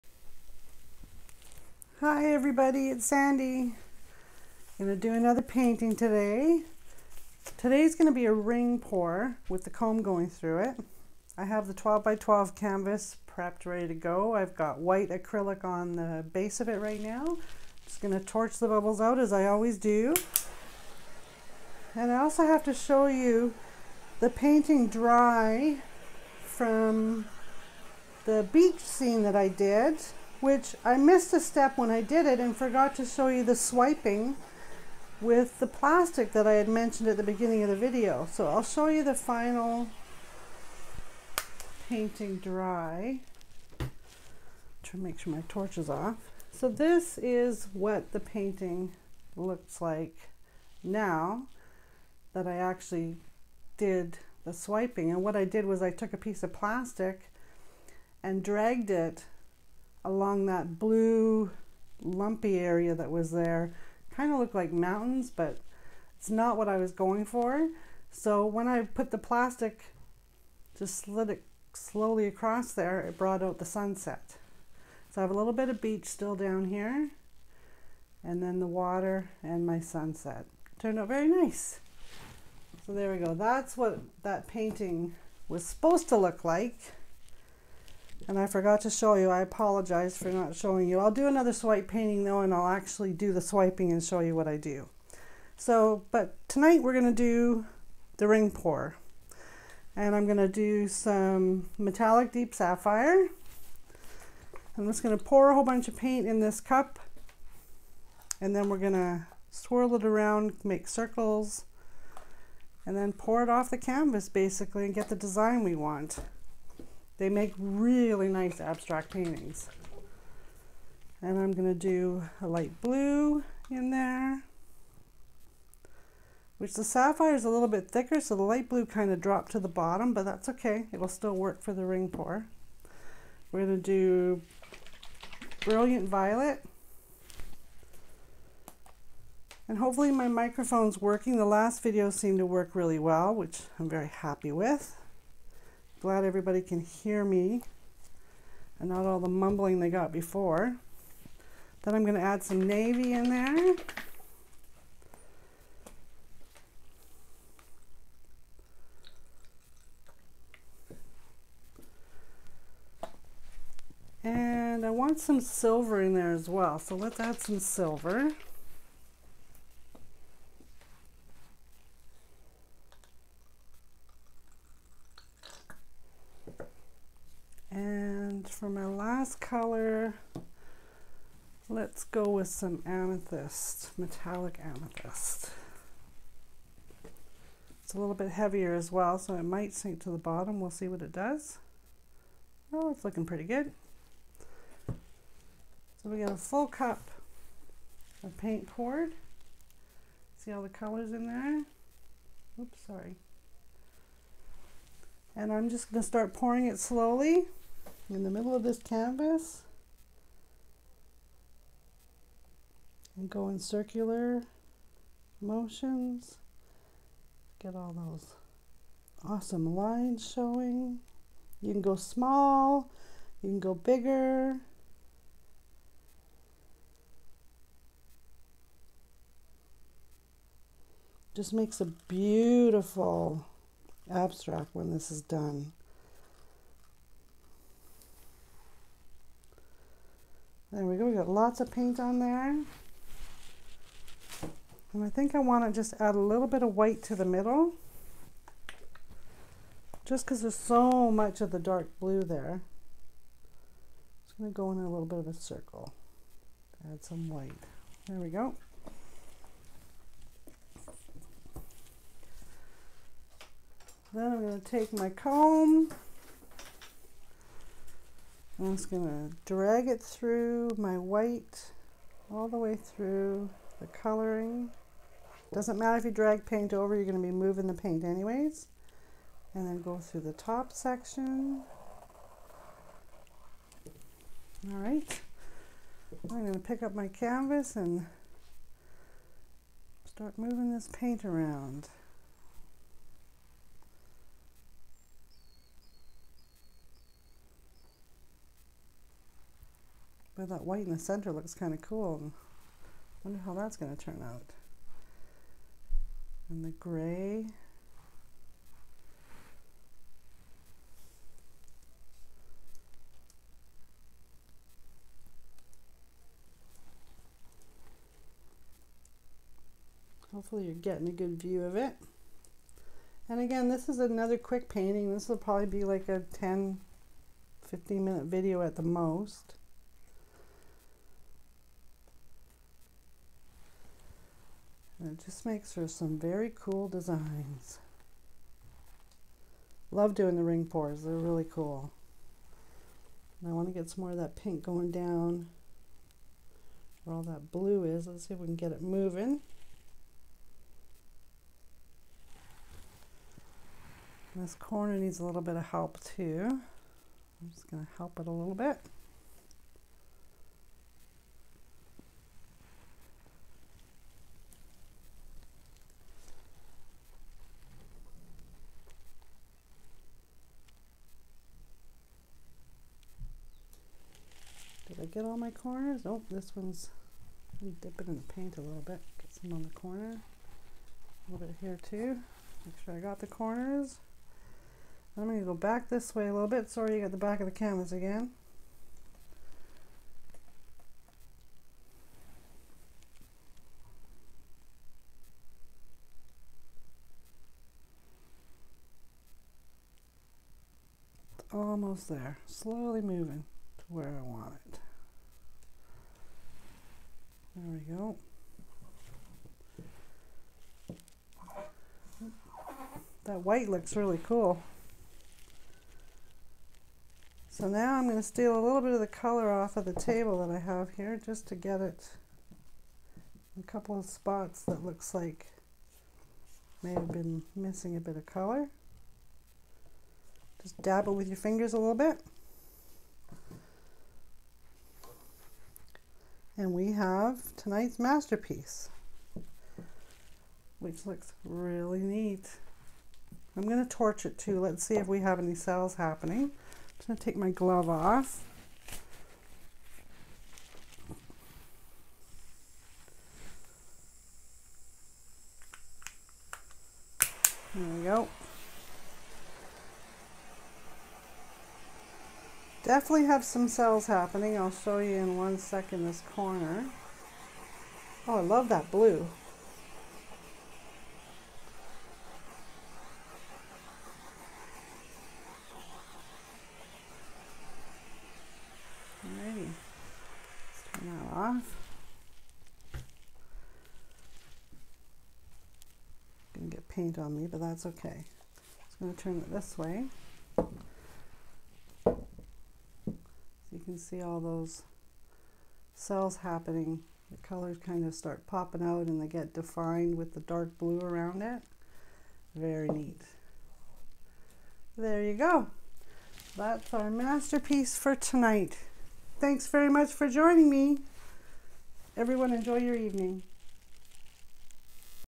Fluid Art/ Acrylic Pour / Comb / 8 minute Art / Easy painting
Fluid Art/ Acrylic Pour / Comb / 8 minute Art / Easy painting - We all have a artist within us all you need to do is try it. Please enjoy and subscribe its free to do and means the world to me because it says to me ... hey I like what your doing 😁👍 comments are always read and welcomed